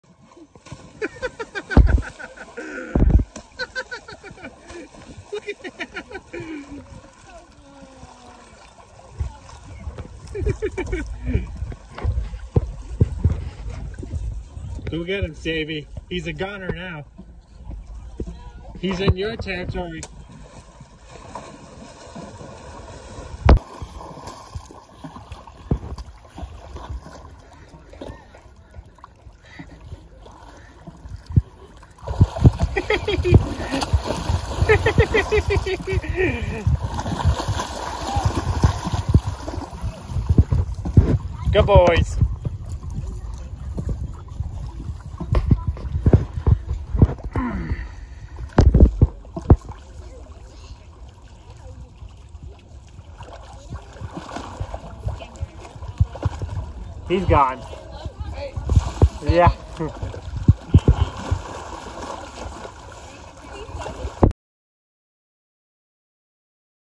Tiger Belly Flop
Watch how Saviour the majestic sea horse attacks (plays rough) his brother Samson as he slipped and fell into Saviours domain!